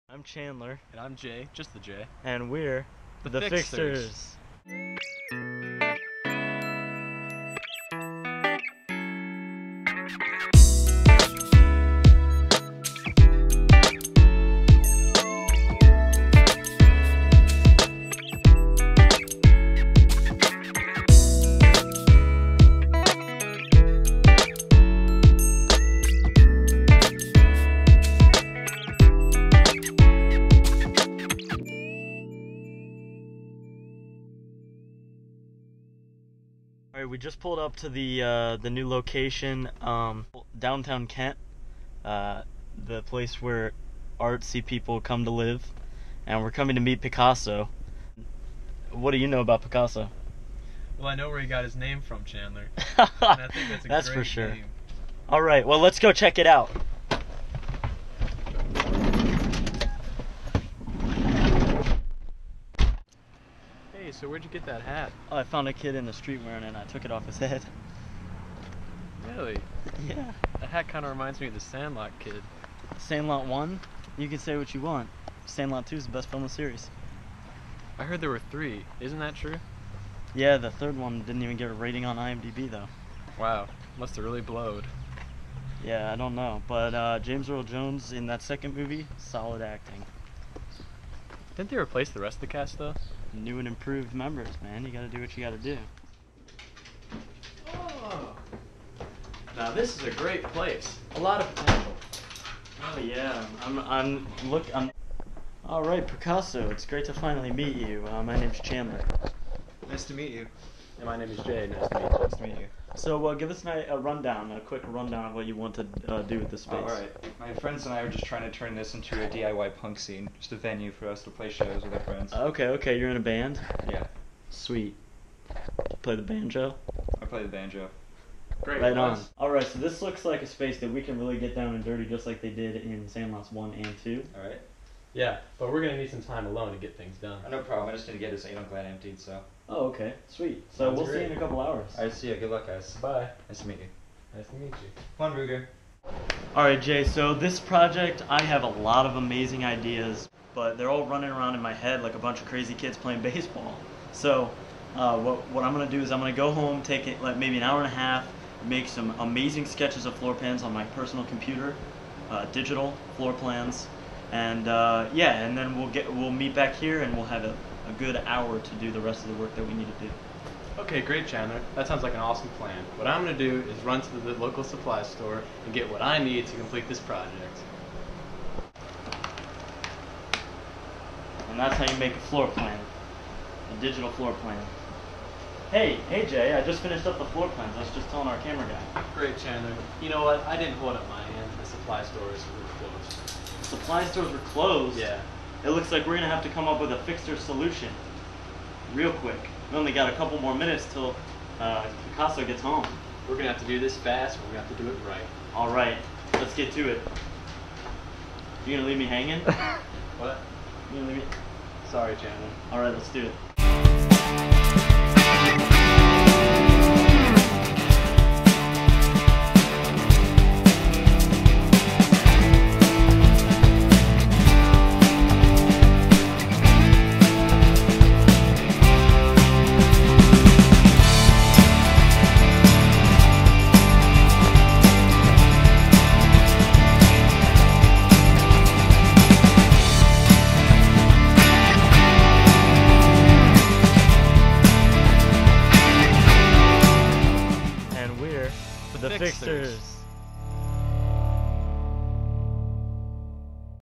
The Fixters
The Pilot episode of new Home Improvement Channel's series "The Fixters"! Chandler and J take on the challenge of a lifetime fixing up Picasso's sweet pad.